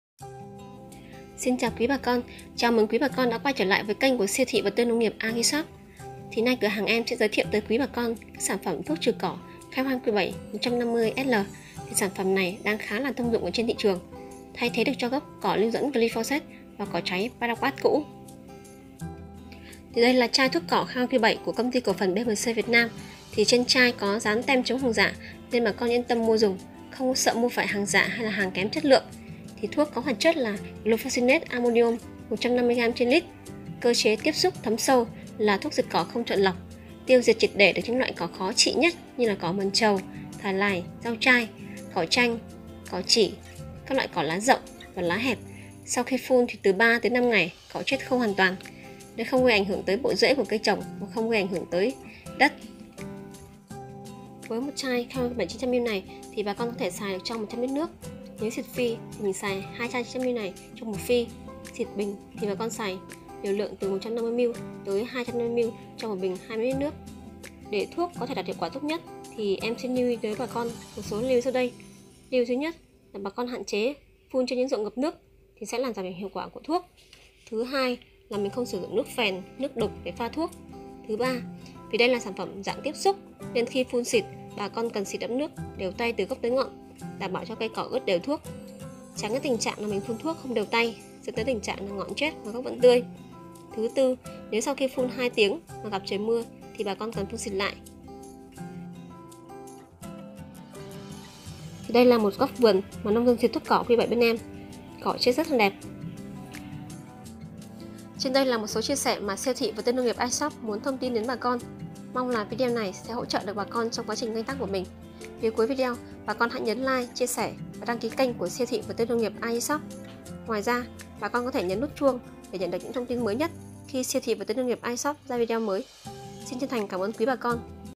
Thuốc trừ cỏ Khai hoang Q7# Nông nghiệp Agrishop
Thuốc trừ cỏ thế hệ mới Khai hoang Q7 ,diệt trừ các loại cỏ tranh ,cỏ ấu ,cỏ chỉ ,các loại cỏ khó trị ,kháng thuốc ,cỏ chết vàng sau 3-5 ngày phun xịt
Giao hàng ship code toàn quốc ,giá bao rẻ